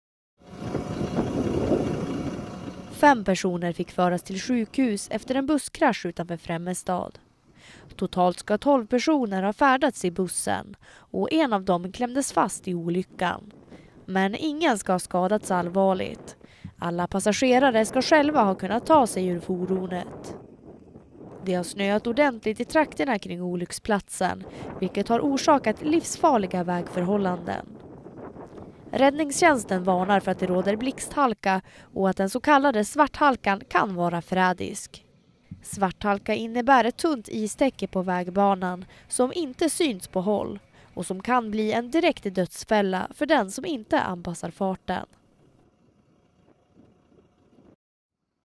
Buss i krasch utanför Främmestad – fem personer till sjukhus
Linjebuss kraschade och välte – en person klämdes